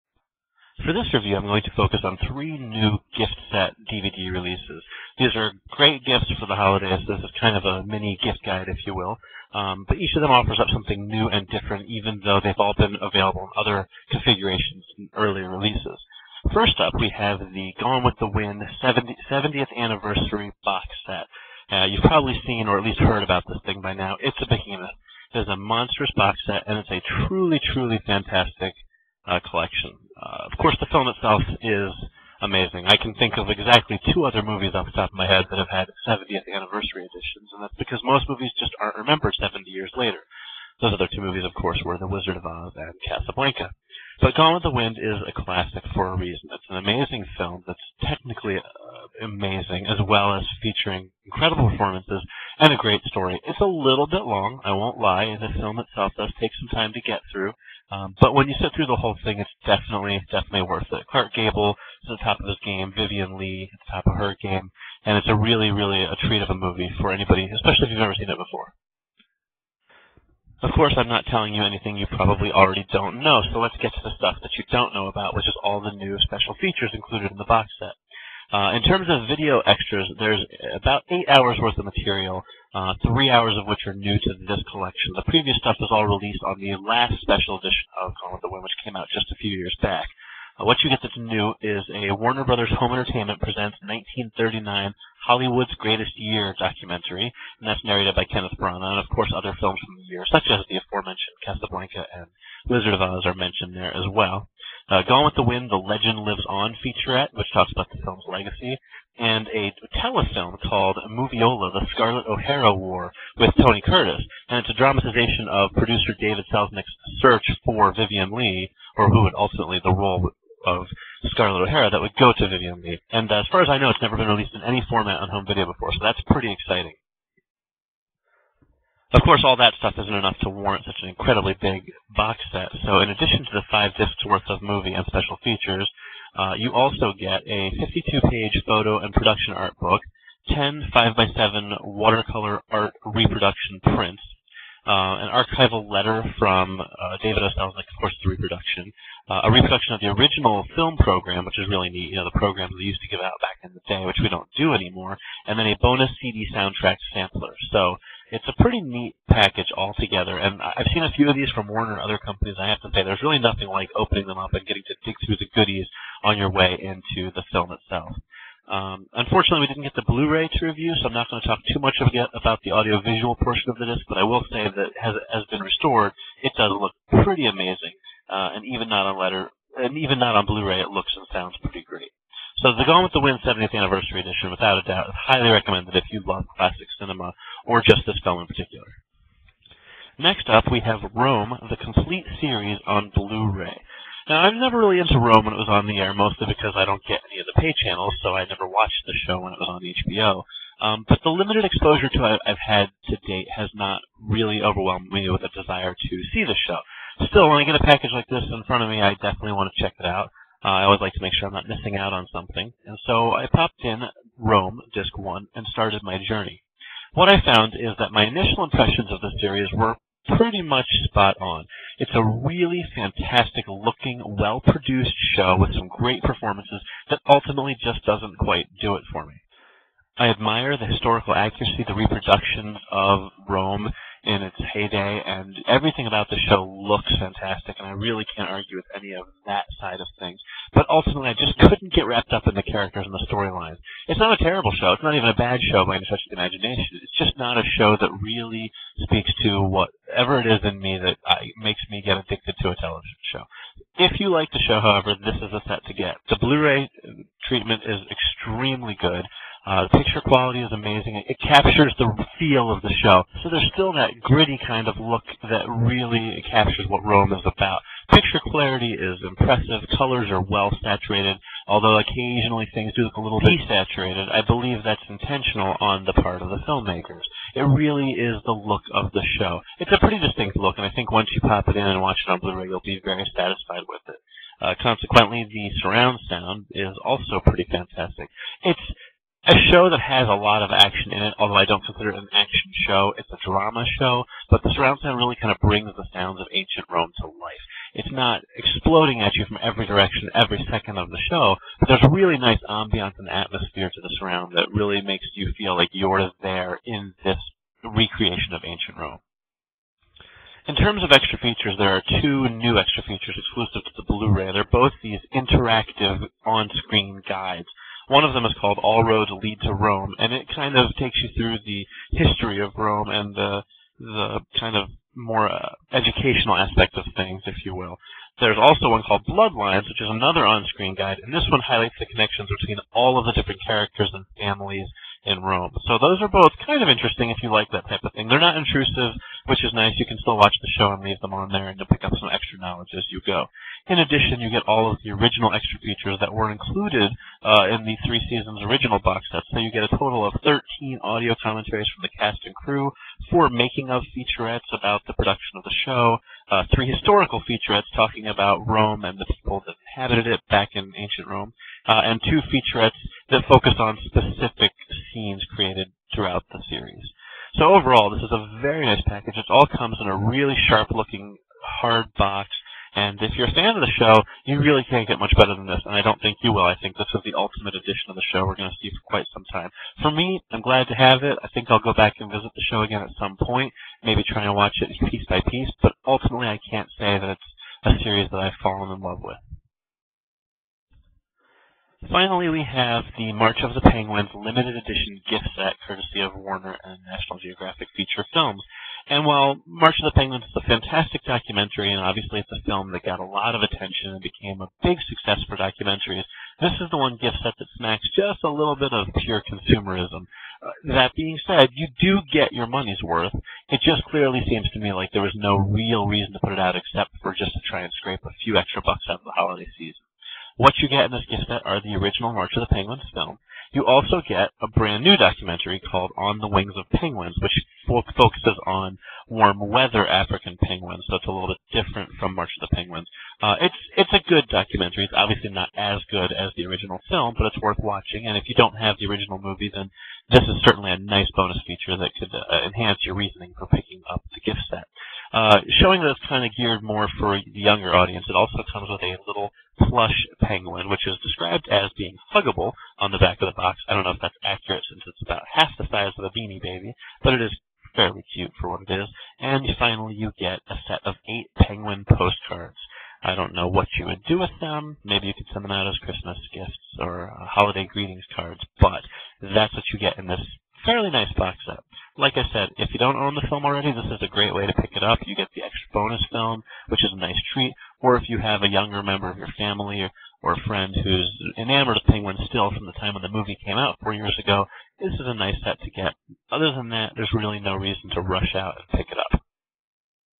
Gone With The Wind, Rome, March of the Penguins
DVD Snapshot's Review of Gone With The Wind: 70th Anniversary Edition, Rome: The Complete Series (Blu-ray), and March of the Penguins: Limited Edition Box Set! For more DVD and Blu-ray reviews, news, giveaways and related interviews,